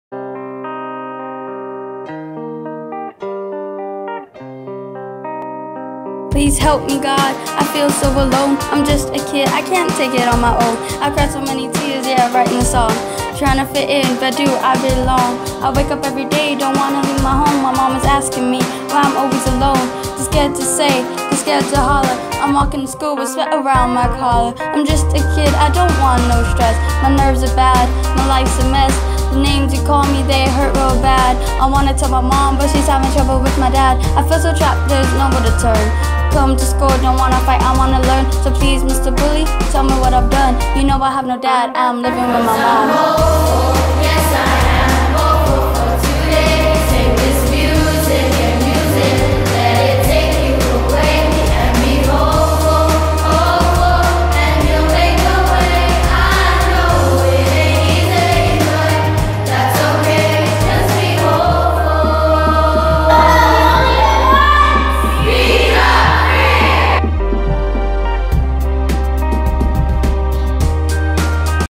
LP Untalan Middle School 2016 YOLO Red Ribbon Week
This was our submission for the 2016 Red Ribbon Week Video contest among several schools on island from the Drama and Choir classes. Original rap was written by the students. We won 2nd place!